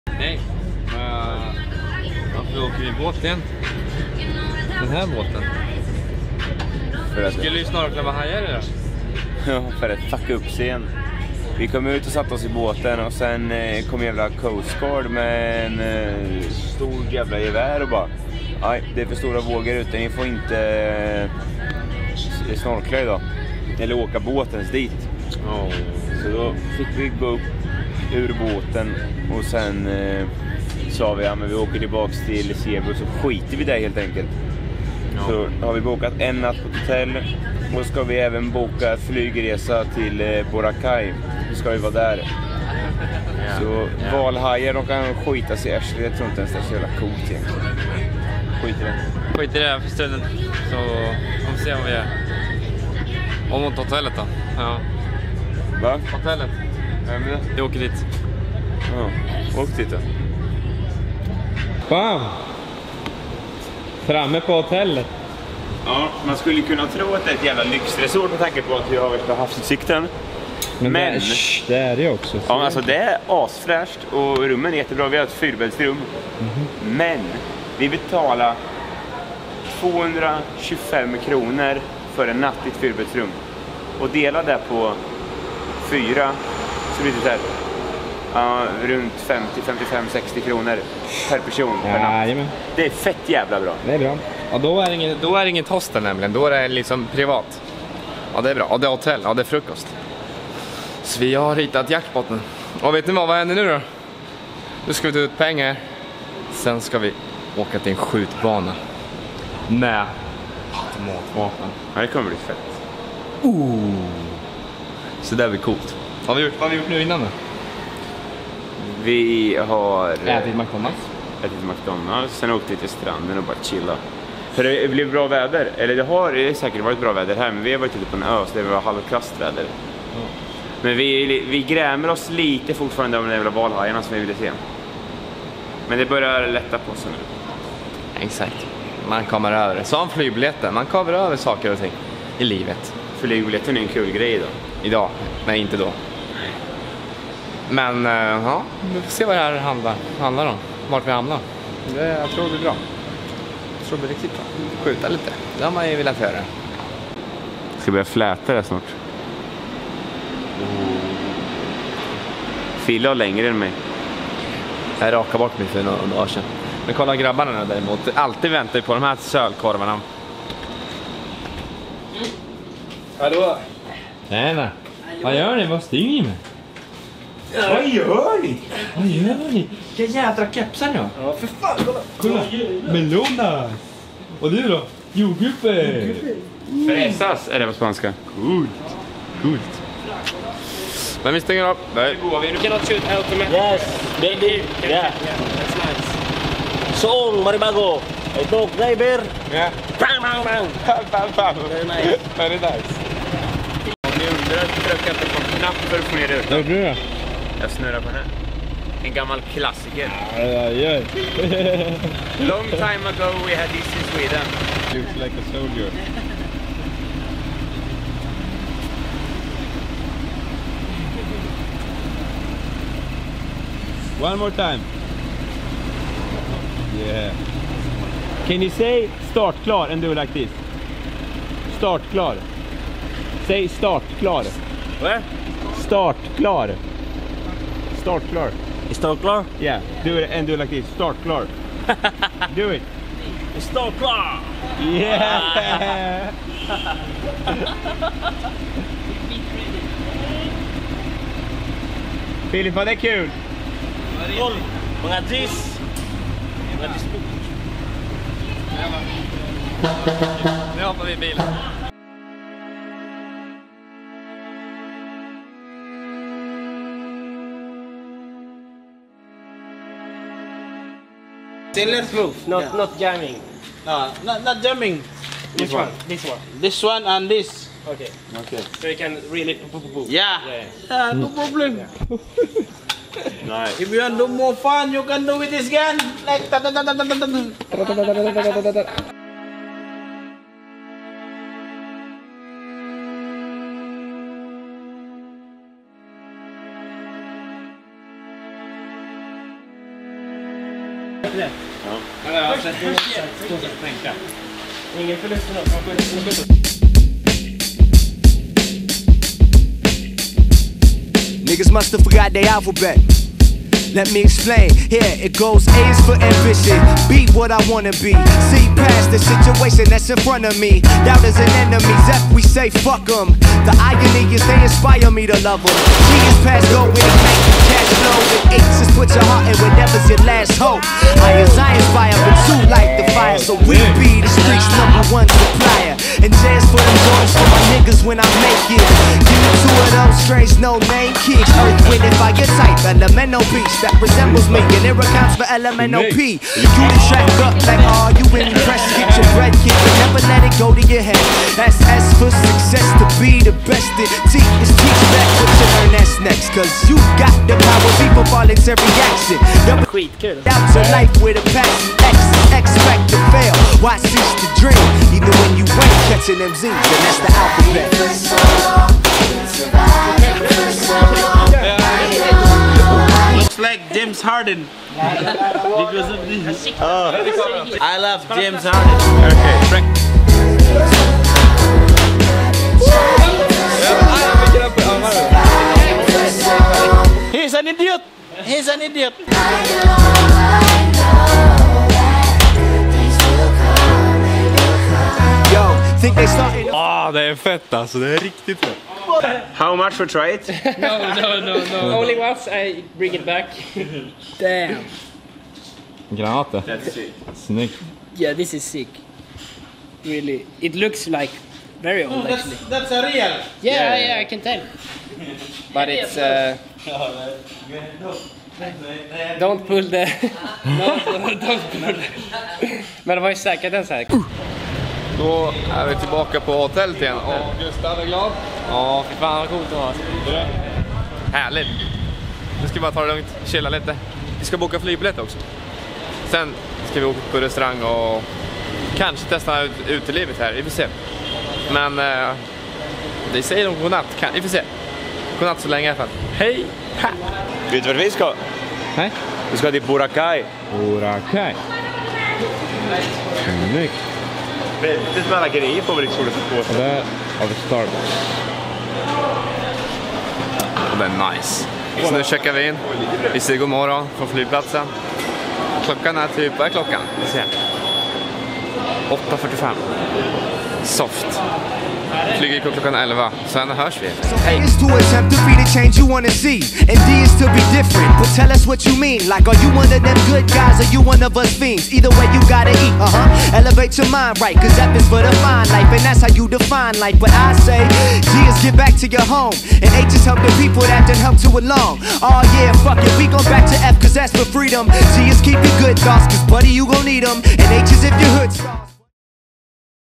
TESTAR SKJUTA AK-2000 | VLOG 18
Automatvapen är olagligt i Sverige. Då testar vi på att testa skjuta det på skjutbana i Filippinerna.

(Råka lägga ut vlog 17 före 18. Därför blir pop-up texten fel i videon)